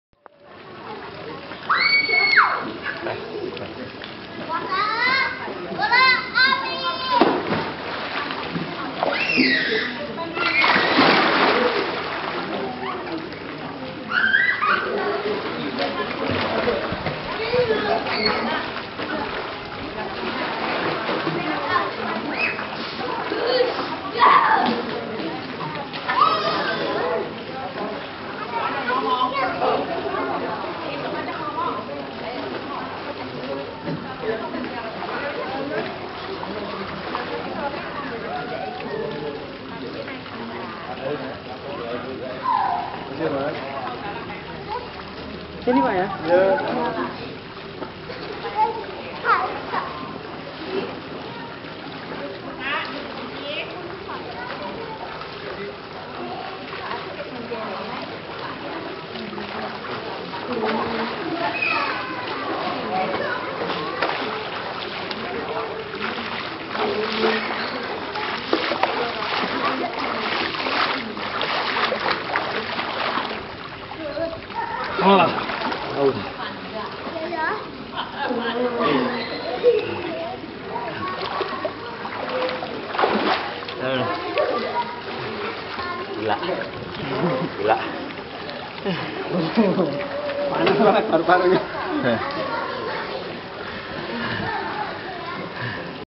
Renang membuat awet muda
Renang membuat awet muda di Fave Hotel Swimming Pool Rembang  sambil pameran arkeologi dan launching rumah peradaban Plawangan Kab. Rembang. Semoga Rumah Peradaban Plawangan menjadi dikenal bagi masyarakat Rembang khususnya  bagi kalangan akademisi, murid sekolah dan masyarakat umum.